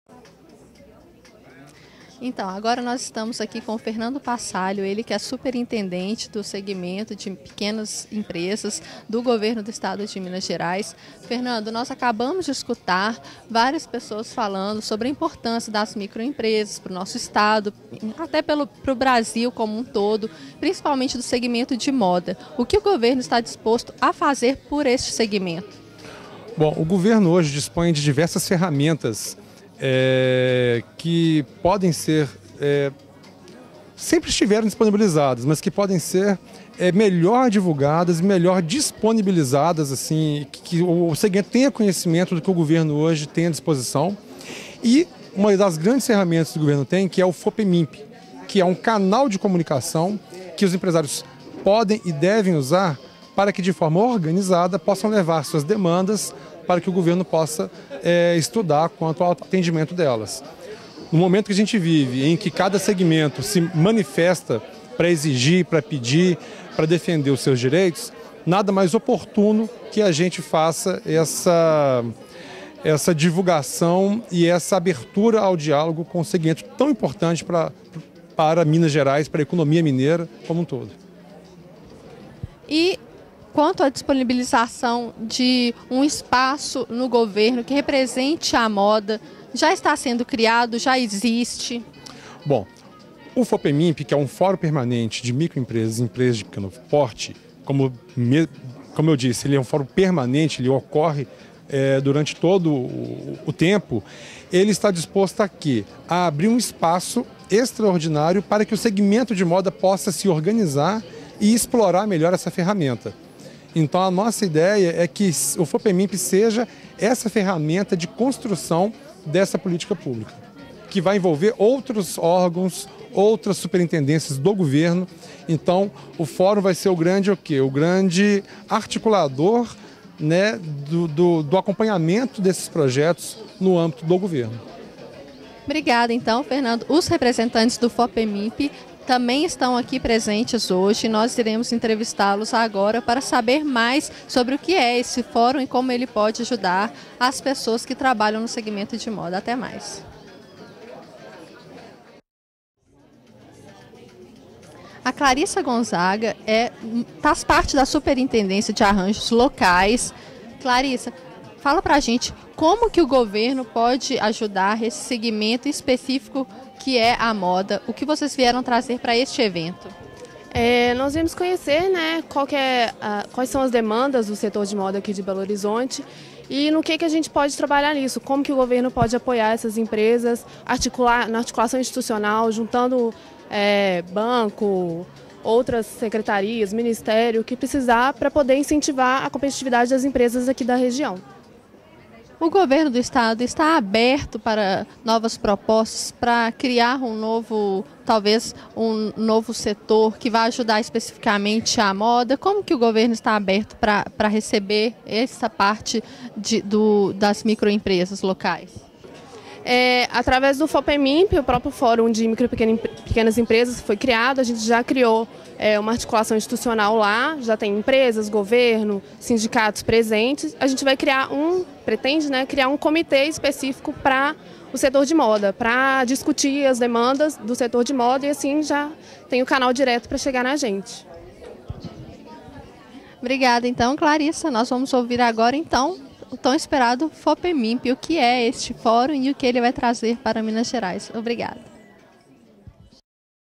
O GOVERNO DO ESTADO DE MINAS GERAIS através dos seus enviados:
FERNANDO PASSÁLIO E CLARISSA GONZAGA declaram apoio ao Conselho criado pelos confeccionistas de BH para gerir o fortalecimento da cadeia produtiva moda. assista as imagens do concorrido almoço/evento realizado no restaurante "Studio Verano" no dia 5/jul/2013.